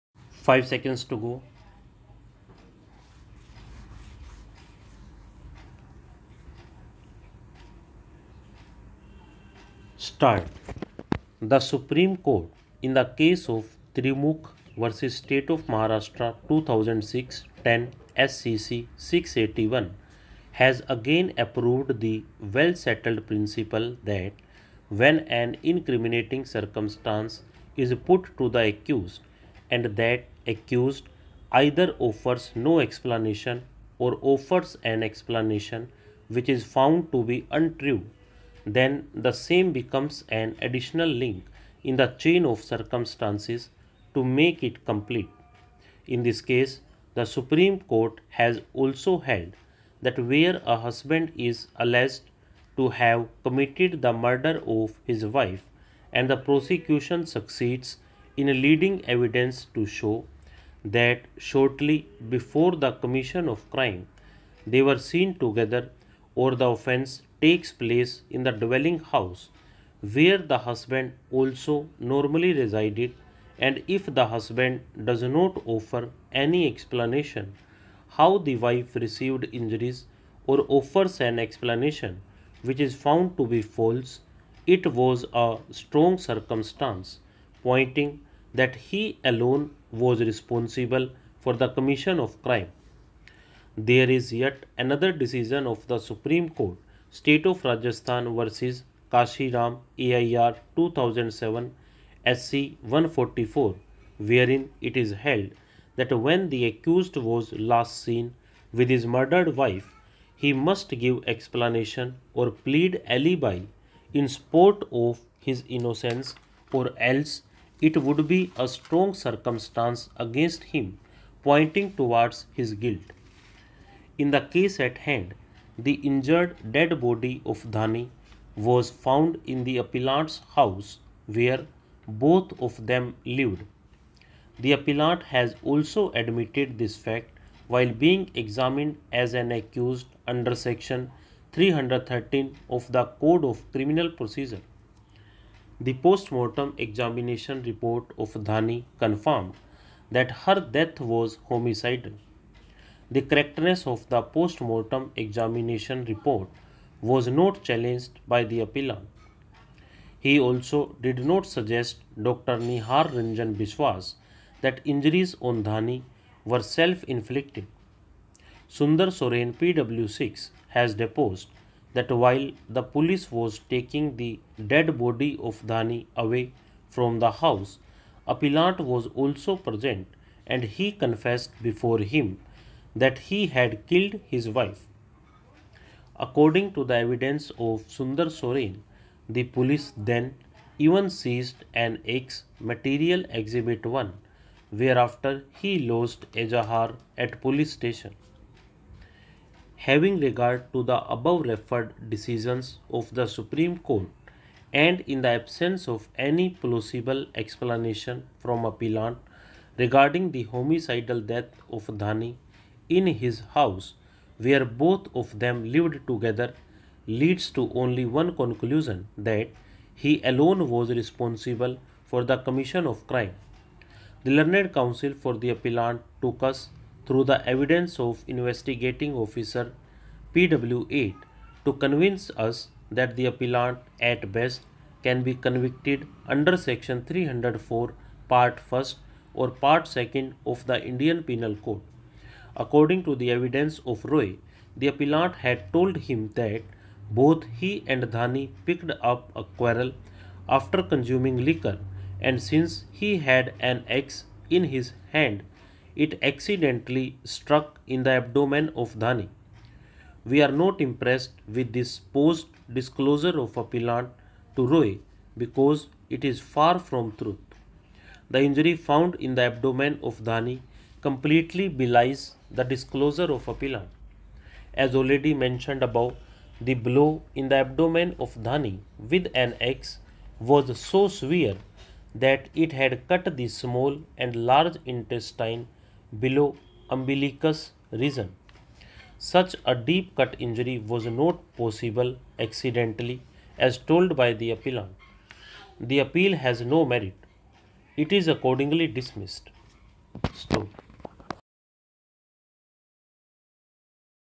120 WPM | English Shorthand | High Court Judgments | Mongal Soren vs. Assam (2)
English Shorthand Dictations | High Court Judgments | Legal Matters

Gauhati High Court Judgment dated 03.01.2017 titled Mongal Soren vs. The State of Assam